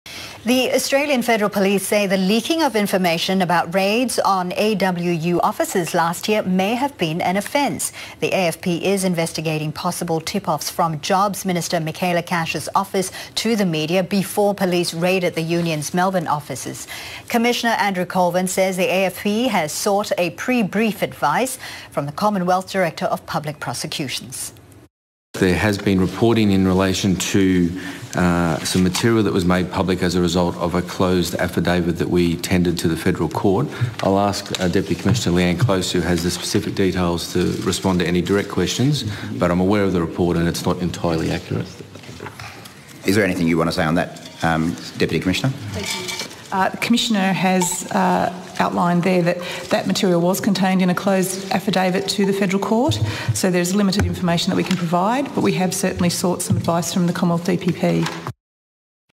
AFP says leaks from Michaelia Cash's office on AWU raids may be an offence
3/08/2018 AFP Commissioner Andrew Colvin confirms advice sought from the DPP on the matter of leaks from Michaelia Cash's office on the AWU raids.